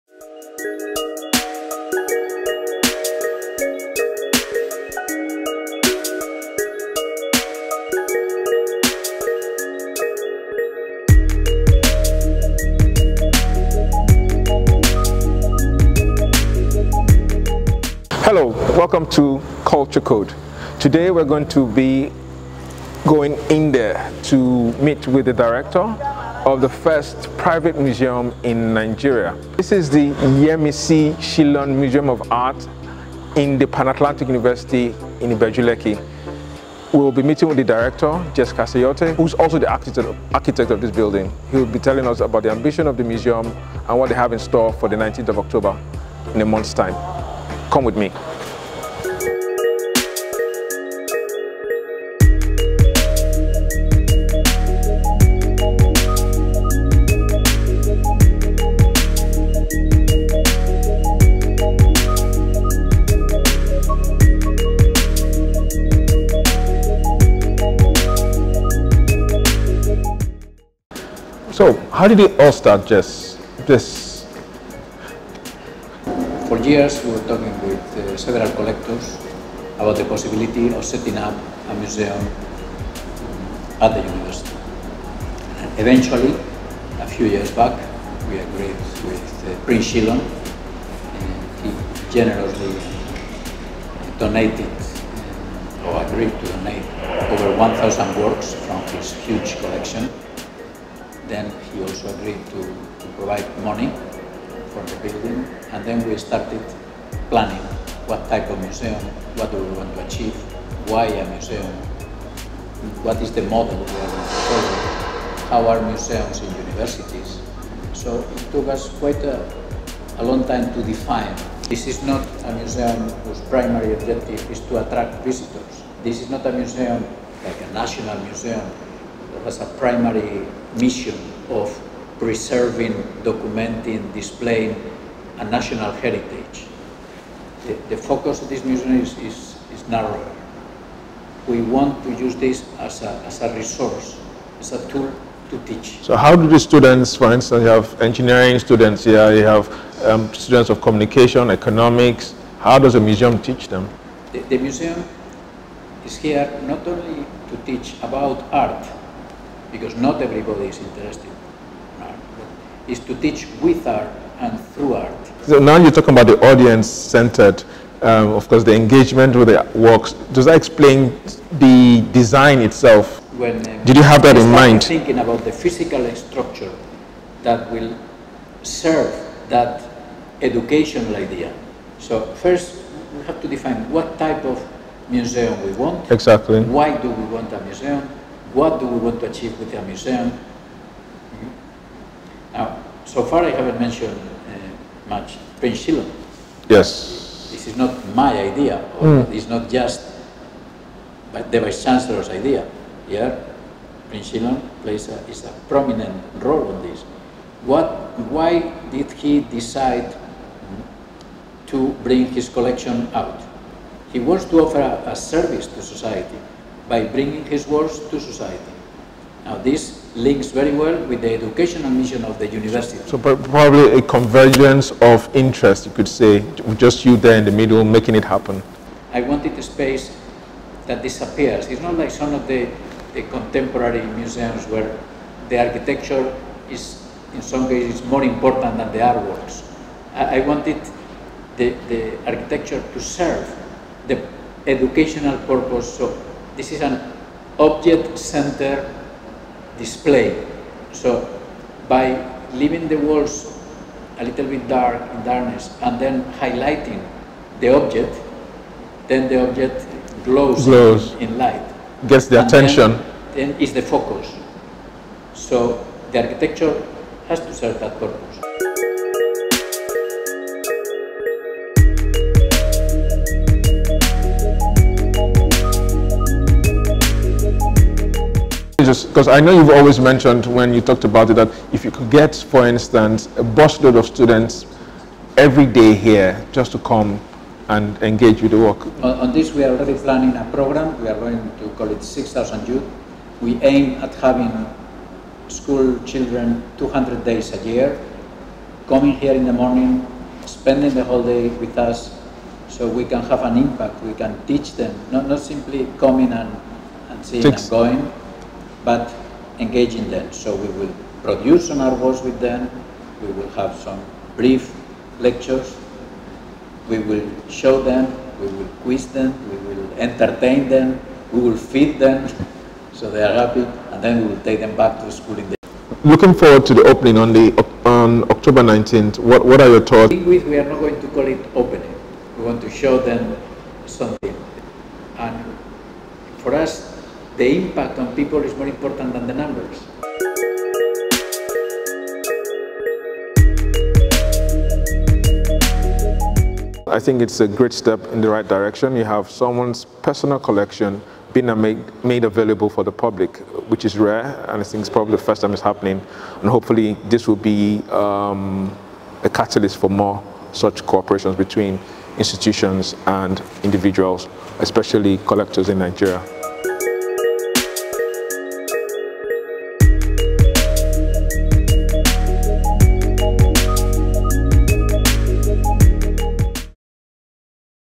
Culture Code: The First Museum in Nigeria , The Yemisi Shyllon Art Museum
We go inside Nigeria's first private museum, The Yemisi Shyllon Art Museum at the Pan Atlantic University campus in Ibeju Lekki.

We speak to the Director, Jess Castellote ahead of the opening.

BusinessDay is West Africa's most authoritative business daily, a diversified provider of superior business, financial and management intelligence across all platforms